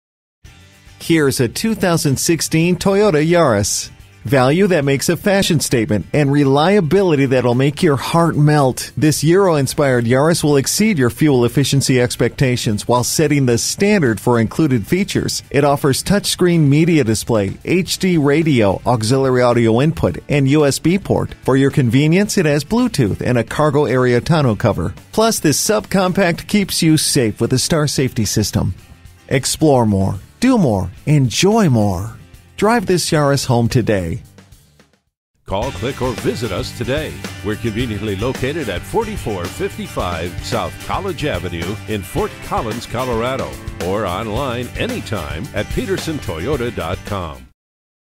Certified 2016 Toyota Yaris Fort Collins Loveland, CO #1830001 - SOLD
Year: 2016
Make: Toyota
Model: Yaris
Trim: L
Engine: 4 Cyl. 1.5 L
Transmission: Automatic
Color: Super White
Interior: Black w/Circle Design
Mileage: 25814
Stock #: 1830001
VIN: VNKJTUD38GA058456
Recent Arrival!CARFAX One-Owner.Certified. Clean CARFAX.

Address:
4455 S. College Ave